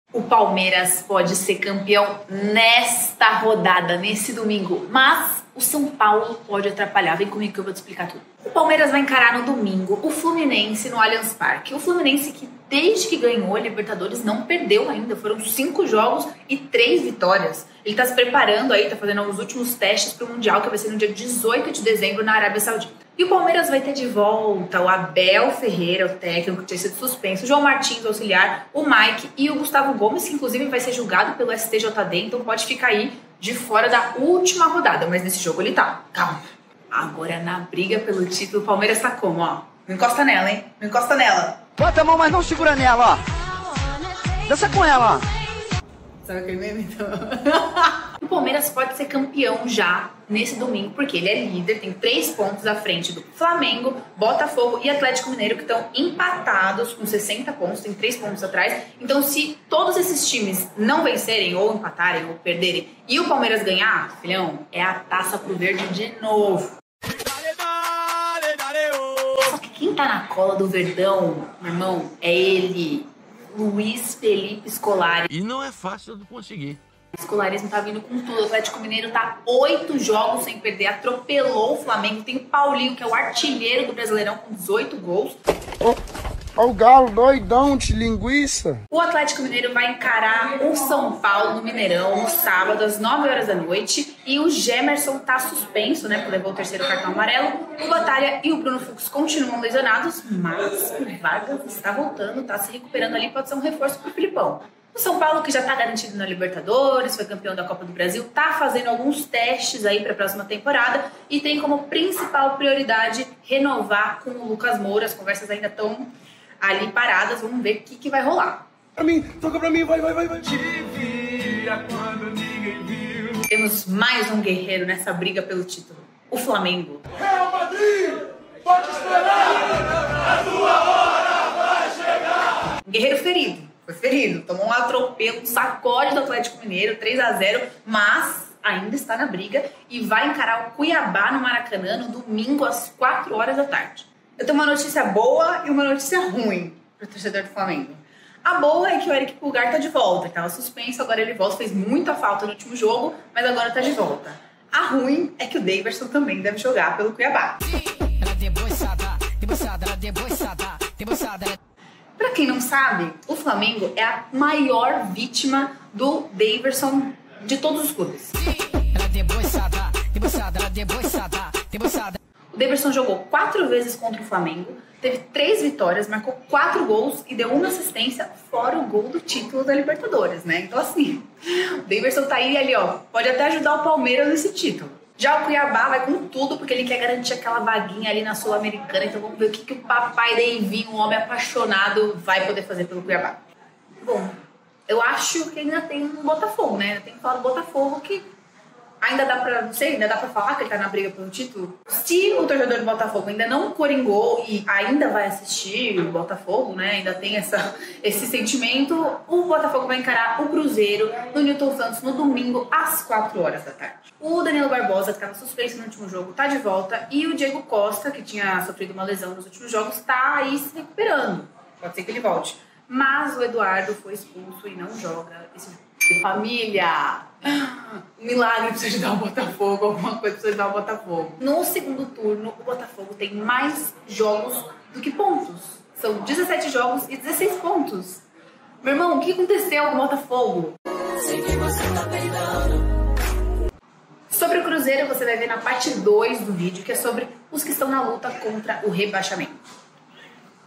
Reta final da Série A: Palmeiras pode ser campeão neste fim de semana | AQUECIMENTO CNN
Depois da pipocada do Fogão, o Palmeiras segue firme - e com vantagem sobre os adversários - na busca pelo dodeca. O time de Abel Ferreira já pode ser campeão neste final de semana.

A reta final da Série A tá alucinante e, mesmo com o Verdão sobrando, Botafogo, Flamengo e Atlético-MG ainda sonham com a taça.

Roteiro e apresentação: Beatriz Consolin
Captação: Lucas Lopes
Edição: Edward Pepe
Arte: Matheus Negrão
Coordenação de produção: Bruna Baddini
Gerência de produto: Veronica Schneider